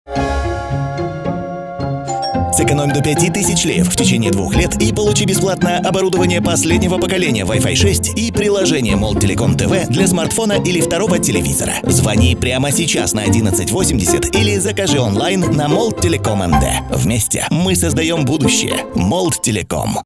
Экономь до 5000 леев и наслаждайся преимуществами
👉 Сэкономь до 5000 леев в течении двух лет и получи бесплатно: оборудование последнего поколения WI-FI 6 и приложение Moldtelecom TV для смартфона или второго телевизора.

📲  Звони прямо сейчас на 1180 или закажи онлайн на moldtelecom.md.

💙 Вместе мы создаем будущее. Moldtelecom.